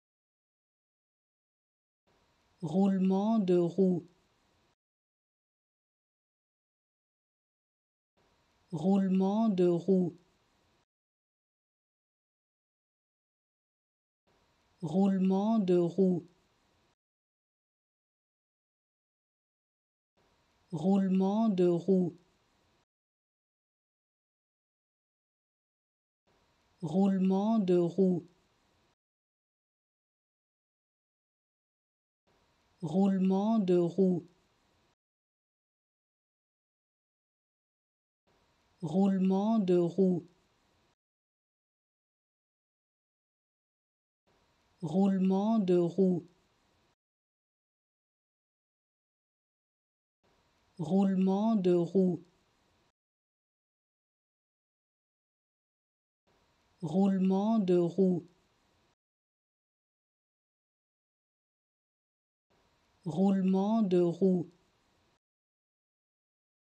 How to Pronounce ''Roulement de roue'' (Wheel bearing) Correctly in French
Learn how to say and properly pronounce ''Roulement de roue'' in French with this free pronunciation tutorial.